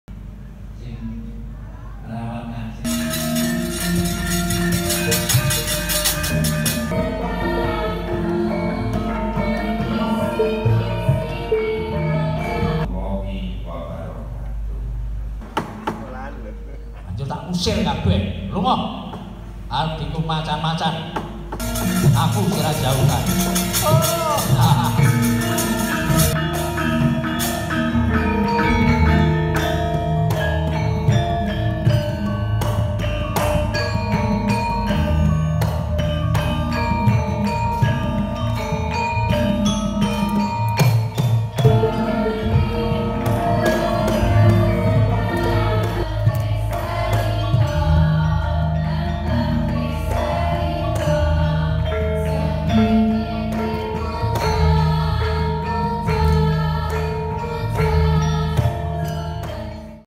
Wayang Kancil
"Wayang Kancil", video dibuat untuk turut serta dalam Lomba Video Ragam Budaya Indonesia, Kuy! IDDiversity yang diselenggarakan oleh Kementerian Pendidikan Kebudayaan Republik Indonesia dan Hellomotion.com. Keterangan lomba